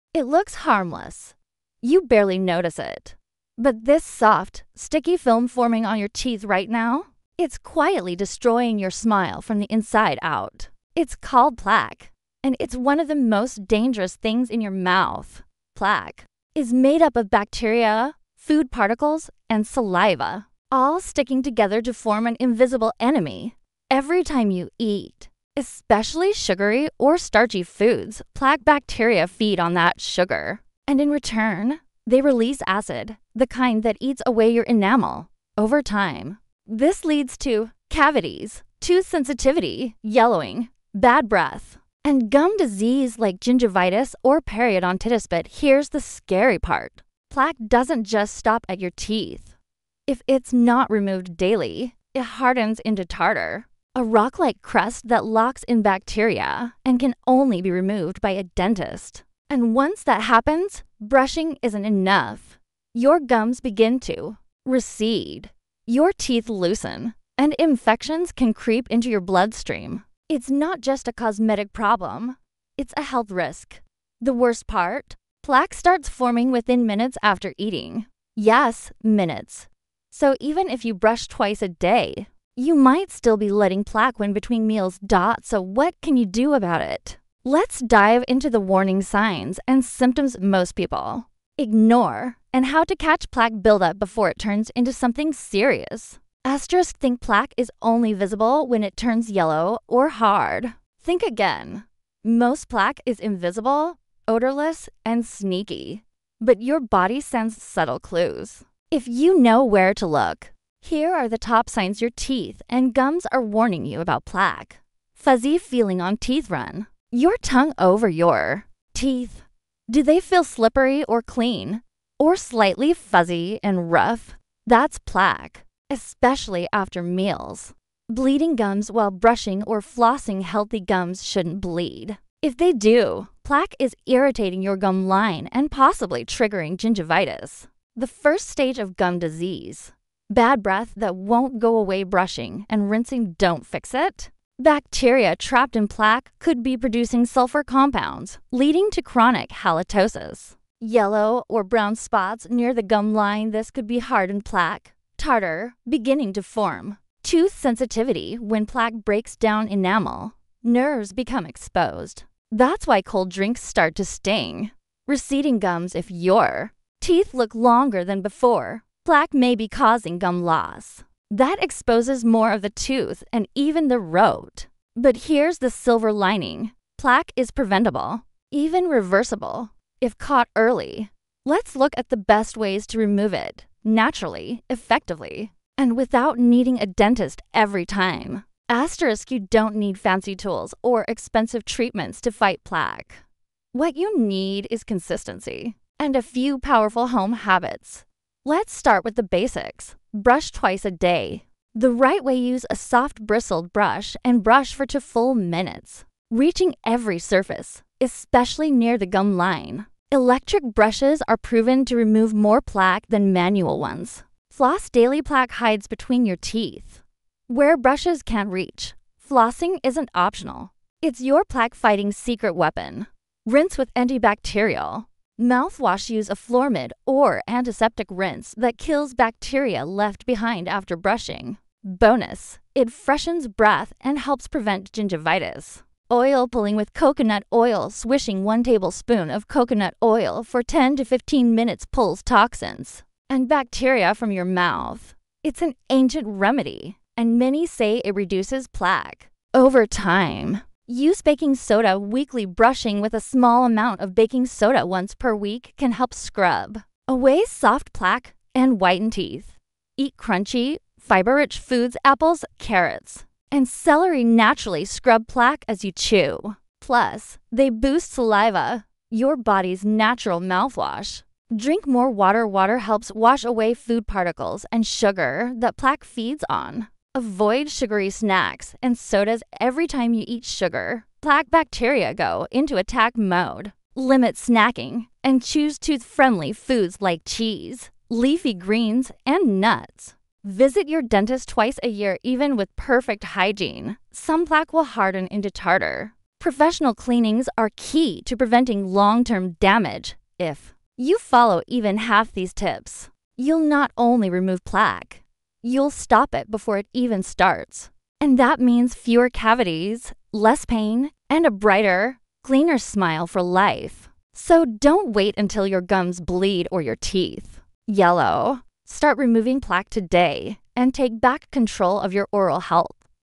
Plaque on Teeth | Clean Plaque on Teeth | Dental Plaque | Dental Plaque Removal
Think your teeth are clean? Think again.
A sticky, invisible film called plaque may be silently destroying your teeth — causing cavities, bad breath, and even gum disease without you knowing it!

In this video, we reveal:
✅ What plaque really is and how fast it forms
✅ Early signs of plaque buildup most people miss
✅ Home remedies and daily tricks to remove it naturally
✅ How to stop plaque before it hardens into tartar
✅ And the one daily habit that could save your teeth!

Whether you're over 40 or just trying to protect your smile, this guide will show you how to take control of your oral health — without expensive dental treatments.

🦷 Clean teeth start with the right knowledge — watch now and protect your smile before it’s too late!